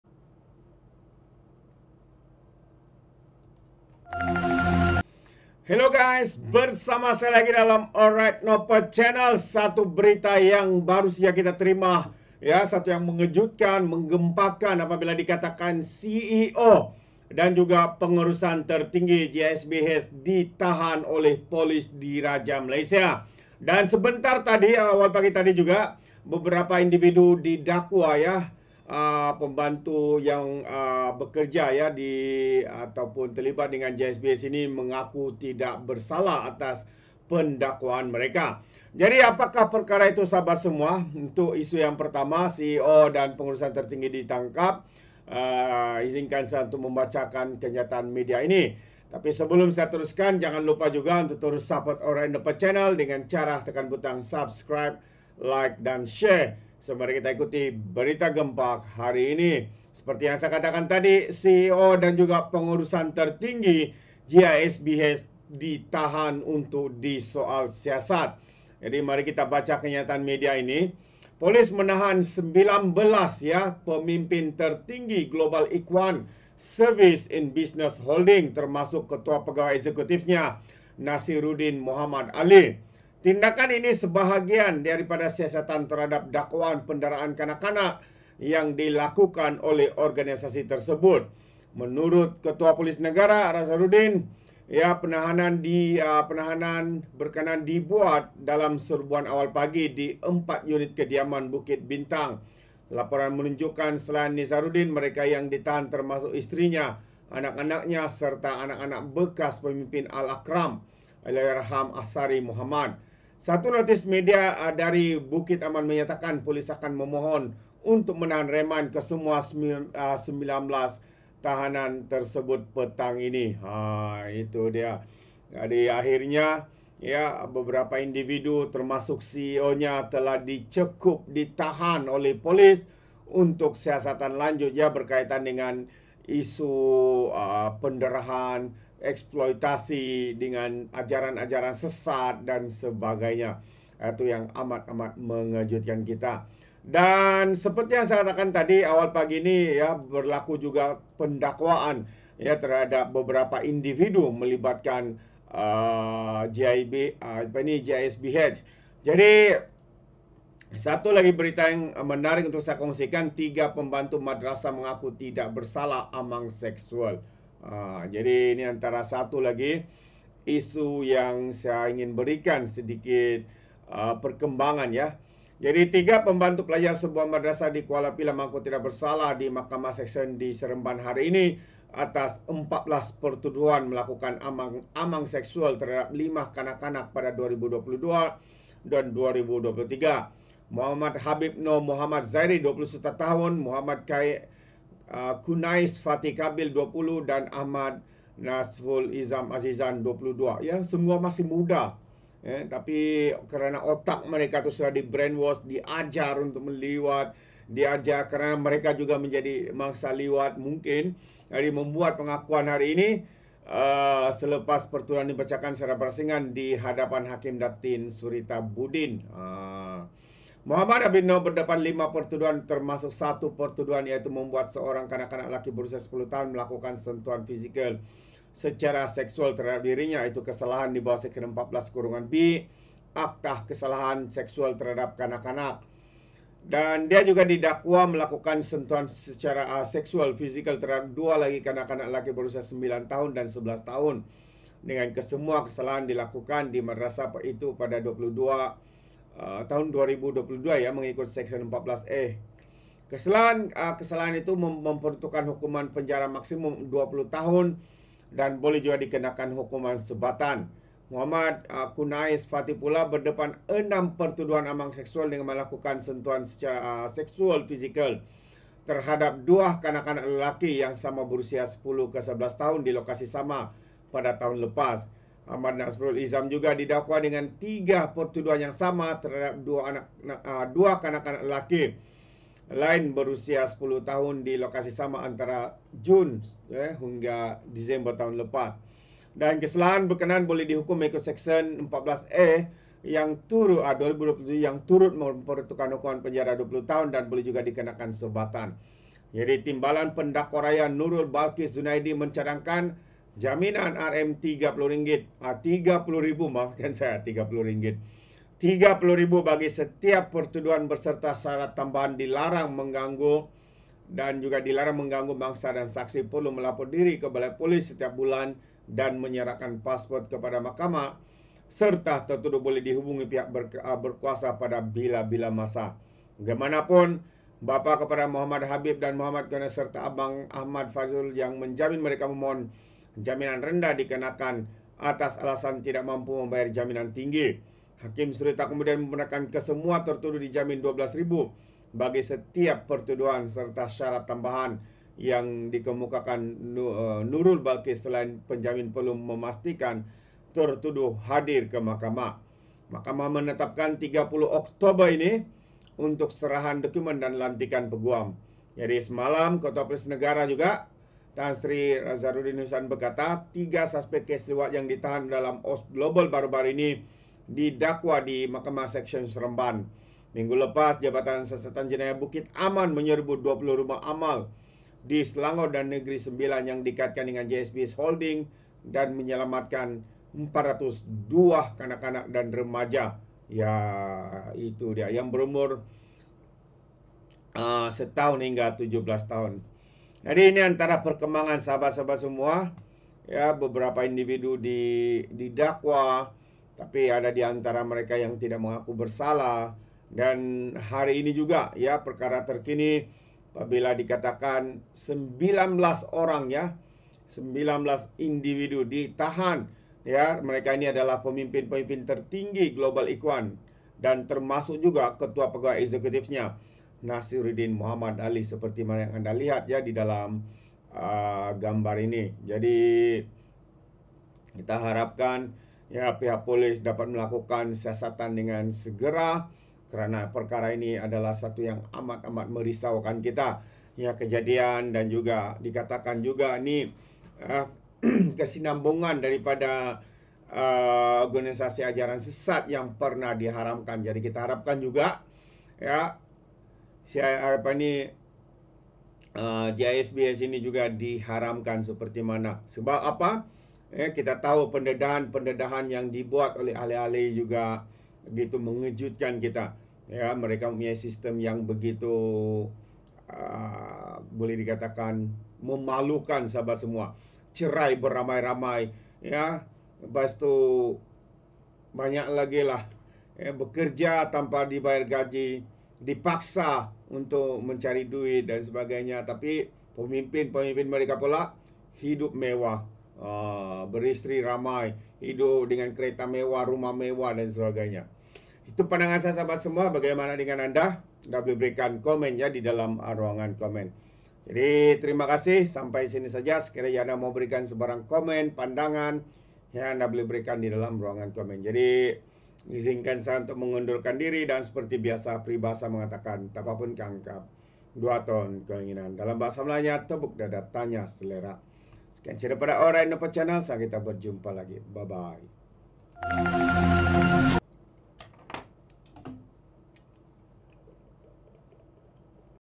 [viral] CEO & PENGURUSAN TERTINGGI GISBH DITAHAN PDRM! PEMBANTU TIDAK MENGAKU DIMAHKAMAH!
Polis menahan 19 pemimpin tertinggi Global Ikhwan Services and Business Holdings (GISBH), termasuk Ketua Pegawai Eksekutifnya Nasiruddin Mohd Ali.
Tindakan ini sebahagian daripada siasatan terhadap dakwaan penderaan kanak-kanak yang berlaku di bawah pengawasan organisasi tersebut.
Ketua Polis Negara Razarudin Husain memaklumkan penahanan itu dibuat dalam serbuan awal pagi di empat unit kediaman di Bukit Bintang.
Laporan menunjukkan selain Nasiruddin, mereka yang ditahan termasuk isterinya, anak-anaknya, serta anak-anak bekas pemimpin al-Arqam, Allahyarham Ashaari Mohammad.